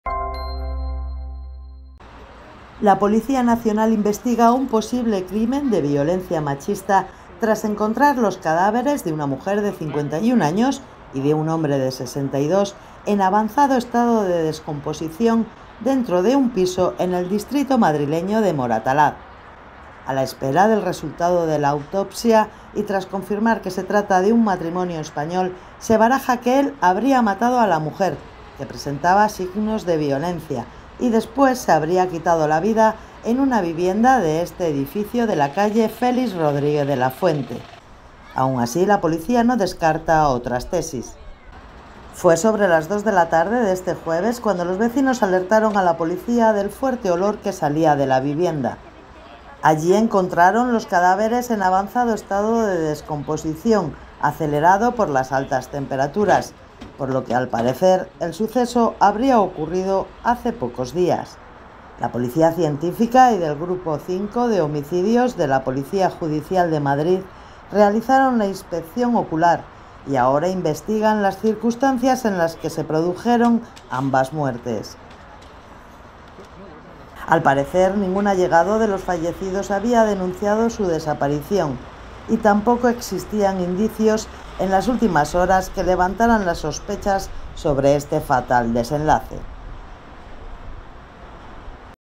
Investigan crimen machista tras encontrar los cadáveres de una pareja en vivienda de Moratalaz
Madrid, 18 ago (EFE).- La Policía Nacional investiga el hallazgo este jueves de los cadáveres en avanzado estado de descomposición de una mujer de 51 años y un hombre de 62 en un domicilio del distrito madrileño de Moratalaz, un suceso en el que se maneja como principal hipótesis que se trate de un nuevo crimen de violencia machista.
Según las primeras averiguaciones, el varón, de nacionalidad española, habría matado a la mujer -aún se desconoce la relación concreta entre ambos- y después se habría quitado la vida, aunque no se descartan otras tesis, informan a EFE fuentes policiales. EFE.
IMÁGENES: RAMÓN AYALA/TATO PÉREZ
IMÁGENES DE LOS EXTERIORES DEL EDIFICIO DONDE SE ENCONTRARON LOS CADÁVERES EN LA CALLE FÉLIX RODRÍGUEZ DE LA FUENTE, EN EL DISTRITO MADRILEÑO DE MORATALAZ.
AGENTES DE POLICÍA, BOMBEROS  Y FURGÓN FUNERARIO.
EDICIÓN Y LOCUCIÓN: ANA BELÉN URDILLO